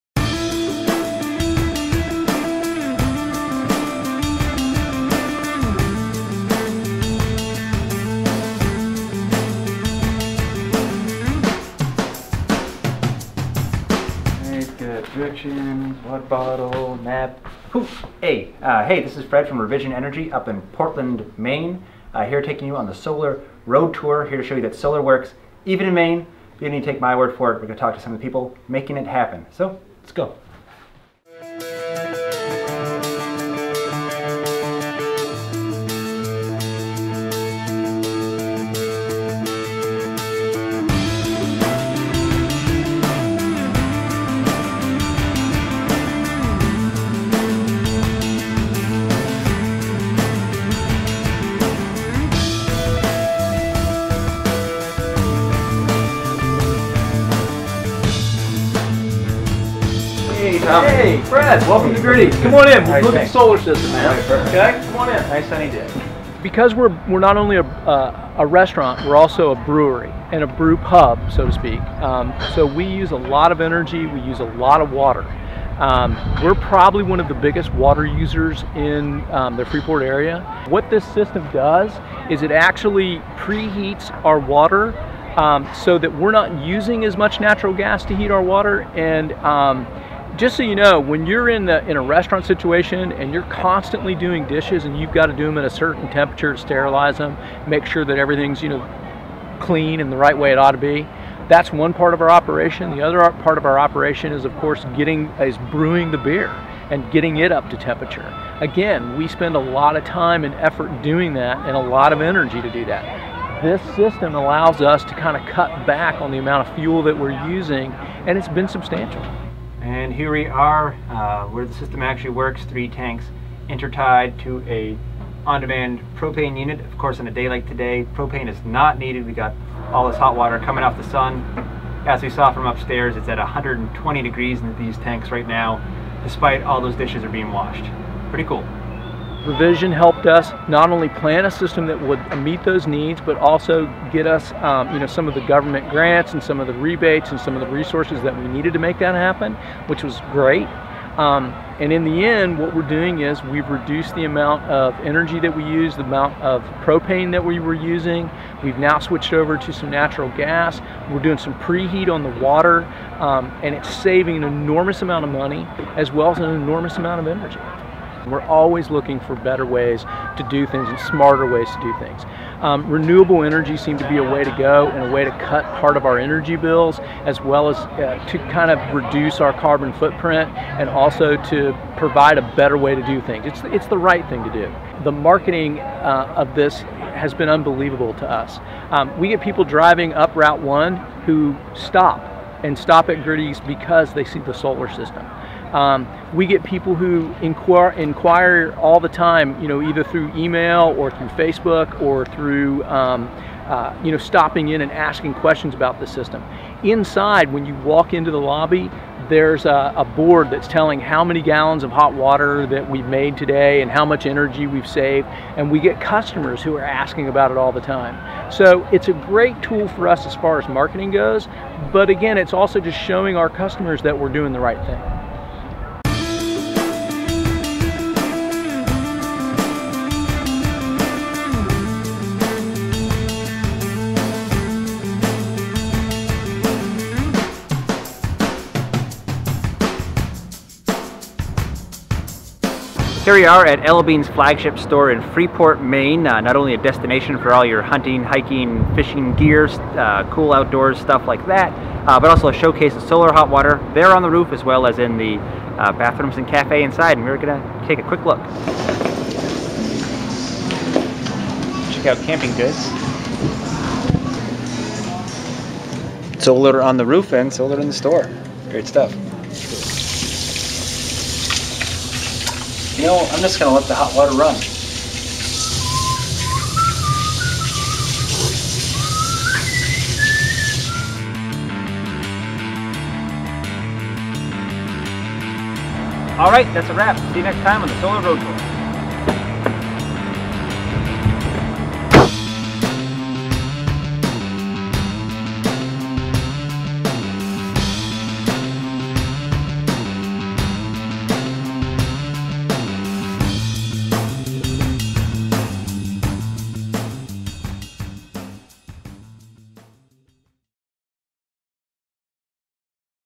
The Solar Road Tour - Ep. 1
Revision Energy - The Solar Road Tour!  Summer 2011, see how solar energy is working in Maine!